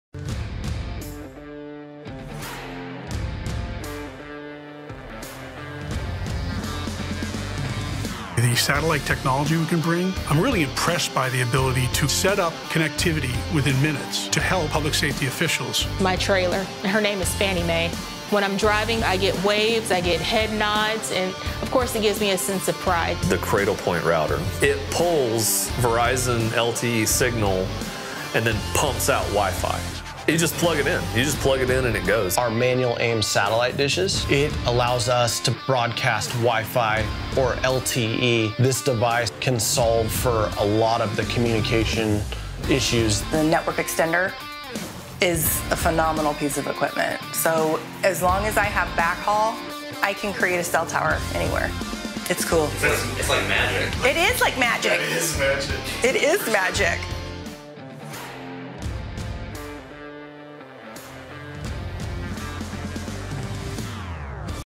Network Equipment for Crisis & Disaster Incidents | Verizon Response Team
The Verizon Response Team is ready to assist with network and cellular equipment for public safety officers during times of need. Public safety agencies all rely on proper communication and interoperable systems. Using Verizon’s 5G network the Verizon Response Team is able to set up and provide cellular network communication to help aid in any situation for any public safety officer.